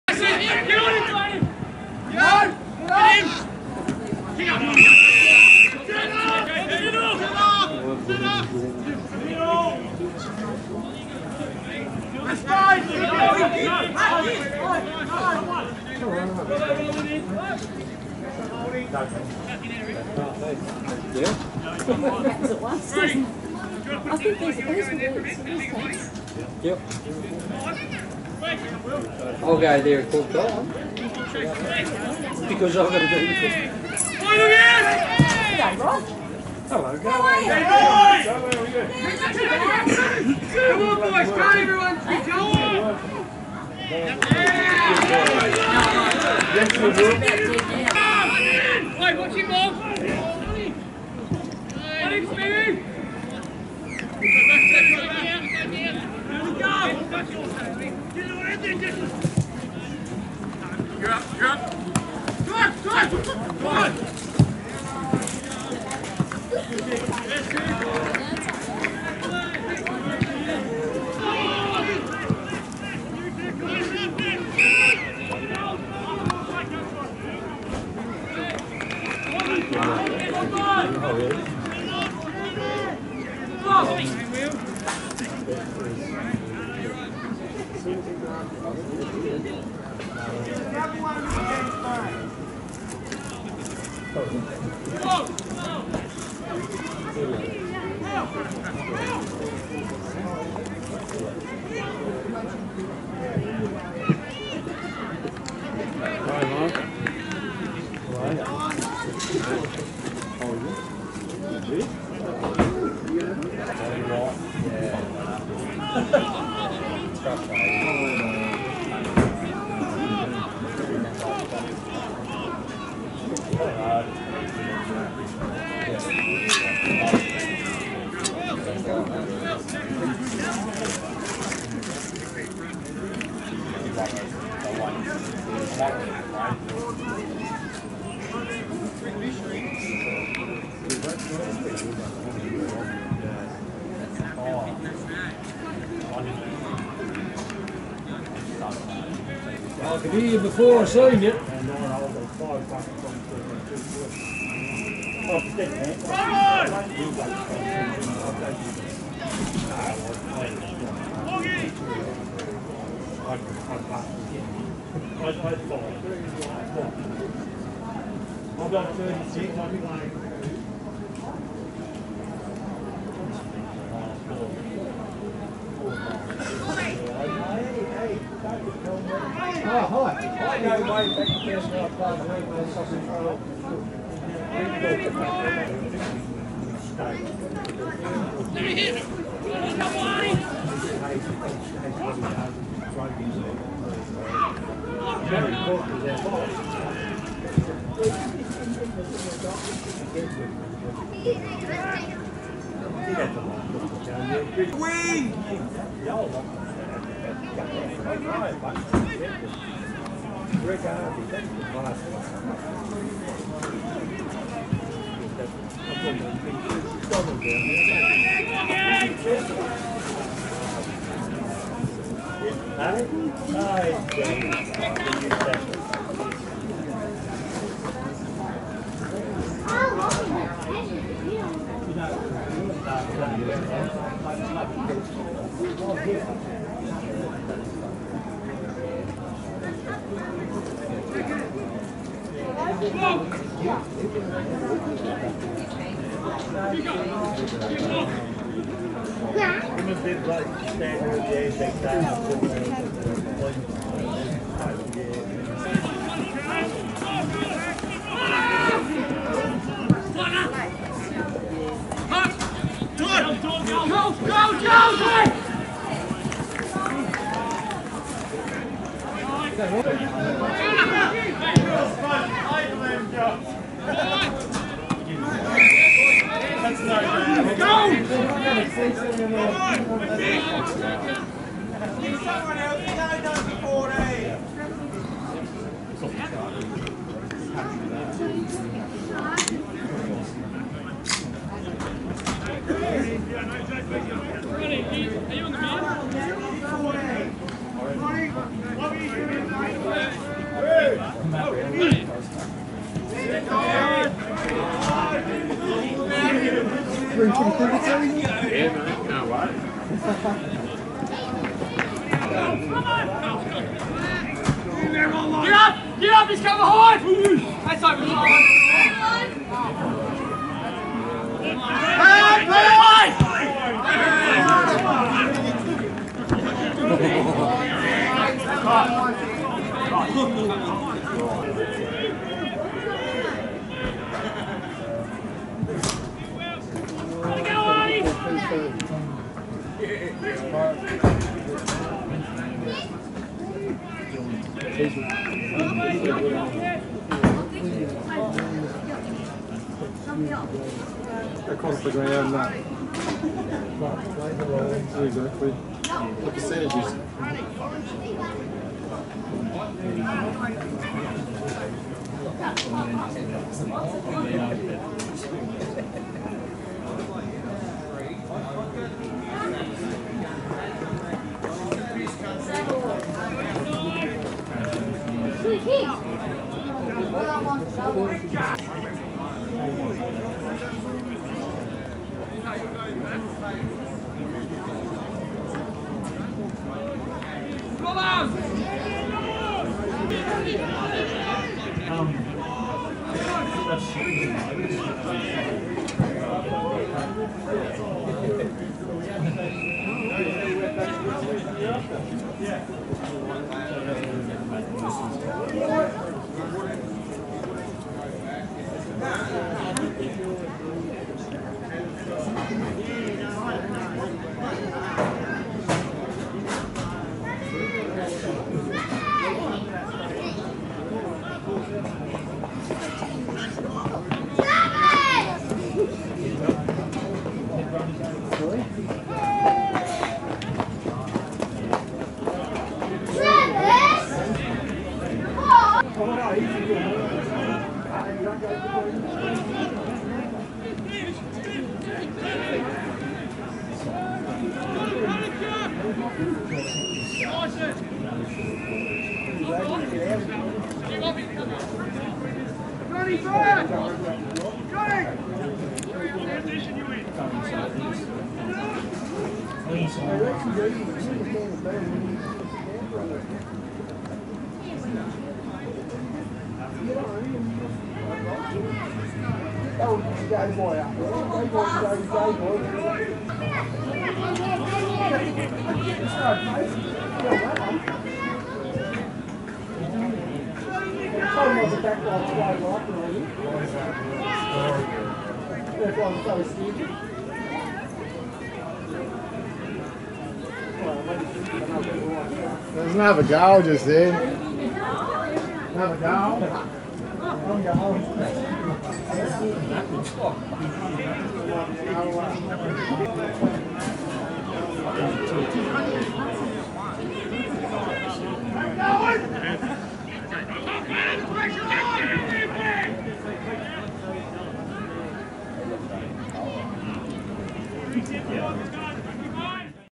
NTFA Boags Shield - Rohan Sergeant 16 goals v University
All of Rohan Sergeant's 16 goals for Bridgenorth against University in the Boags St George Shield, played at University, Saturday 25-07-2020